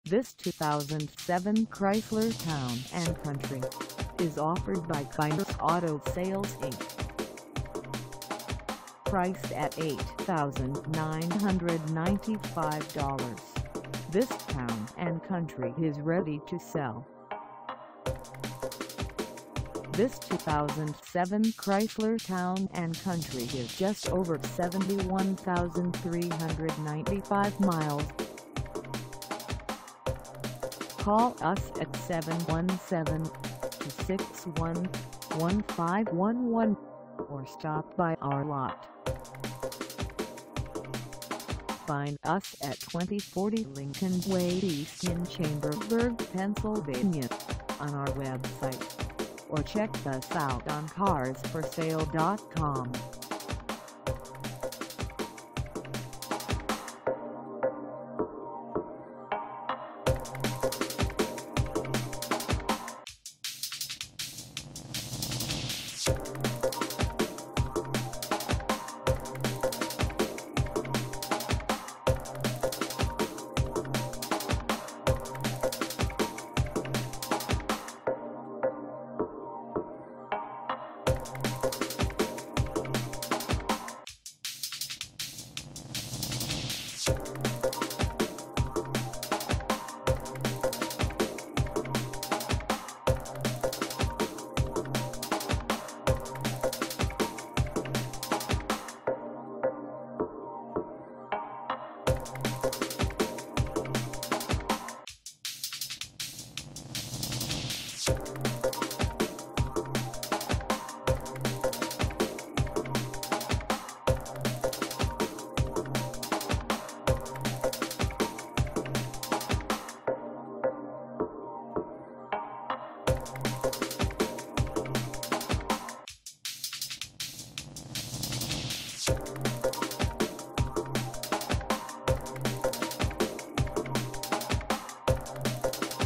2007 Chrysler Town and Country LX 4dr Ext Minivan for sale i
This 2007 Chrysler Town and Country LX 4dr Ext Minivan is for sale in Chambersburg, PA 17202 at Kyners Auto Sales INC.
Engine: 6-Cylinder 3.3L
Transmission: Automatic 4-Speed
Color: Blue
Mileage: 71395
Contact Kyners Auto Sales INC 2040 Lincoln Way E Chambersburg, PA 17202 to test drive this 2007 Chrysler Town and Country LX 4dr Ext Minivan today.